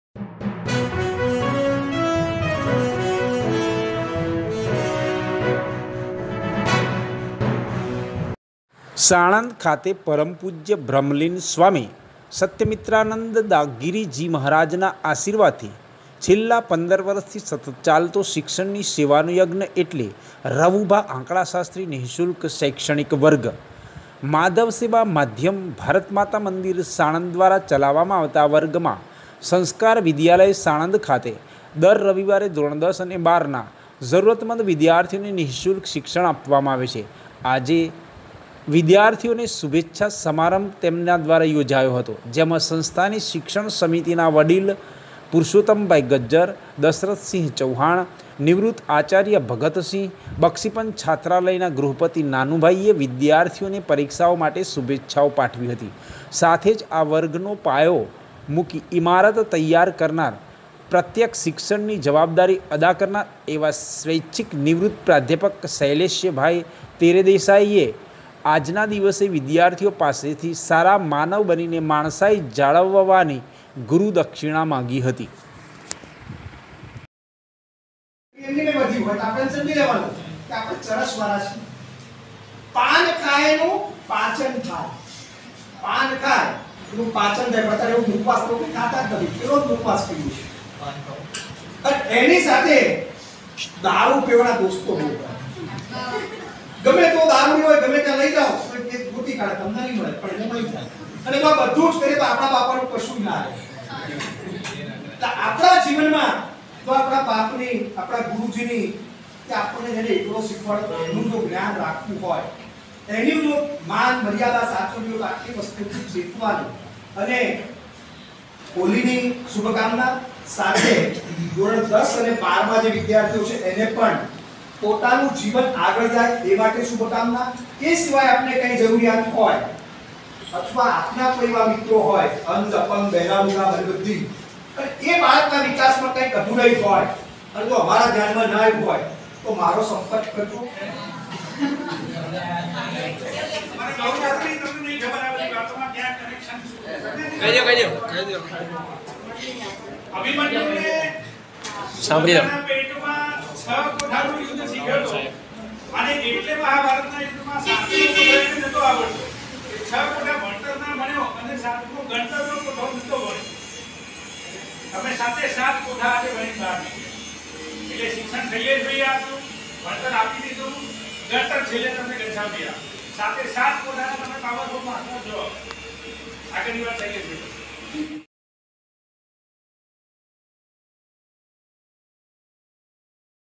સાણંદ ખાતે પ.પૂ. બ્રહ્મલીન સ્વામી સત્યમિત્રાનંદગીરીજી મહારાજના આશીર્વાદથી નિઃશુલ્ક શૈક્ષણિક વર્ગ.
𝗟𝗮𝘁𝗲𝘀𝘁 𝗡𝗲𝘄𝘀
સાણંદ ખાતે પ.પૂ. બ્રહ્મલીન સ્વામી સત્યમિત્રાનંદગીરીજી મહારાજ ના આશીર્વાદ થી છેલ્લા 15 વર્ષ થી સતત ચાલતો શિક્ષણ ની સેવા નો  યજ્ઞ એટલે રવુભા આંકડાશાસ્ત્રી નિઃશુલ્ક શૈક્ષણિક વર્ગ.
માધવ સેવા માધ્યમ, ભારત માતા મંદિર સાણંદ દ્વારા ચલાવવામાં આવતા આ વર્ગ માં સંસ્કાર વિધાલય સાણંદ ખાતે દર રવિવારે ધોરણ 10 તથા 12 ના જરૂરિયાતમંદ વિદ્યાર્થીઓને નિઃશુલ્ક શિક્ષણ આપવામાં આવે છે. આજે દિનાંક 05/03/23 ના રોજ આ વિધાર્થીઓ નો શુભેચ્છા સમારંભ યોજાયો. જેમાં સંસ્થા ની શિક્ષણ સમિતિ ના વડીલ શ્રી પુરૂષોત્તમભાઈ ગજ્જર, દશરથ સિંહ ચૌહાણ, નિવૃત્ત આચાર્ય શ્રી ભગતસિંહ સાહેબ, તથા બક્ષીપંચ છાત્રાલય ના ગૃહપતિ નાનુભાઈ એ વિધાર્થીઓ ને પરિક્ષા માટે શુભેચ્છાઓ પાઠવી હતી, સાથે જ આ વર્ગ નો પાયો મૂકી ઈમારત તૈયાર કરનાર, તથા પ્રત્યક્ષ શિક્ષણ ની જવાબદારી અદા કરનાર એવા સ્વૈચ્છિક નિવૃત્ત પ્રાધ્યાપક શ્રી શૈલેષભાઈ તેરેદેસાઈ એ આજ ના દિવસે વિધાર્થીઓ પાસેથી સારા માનવ બનીને માણસાઈ જાળવવાની ગુરુ દક્ષિણા માંગી હતી.

કેમેરામેન અફઝલ ખાન પઠાણ રિપોર્ટર બહેલોલ મલેક સાણંદ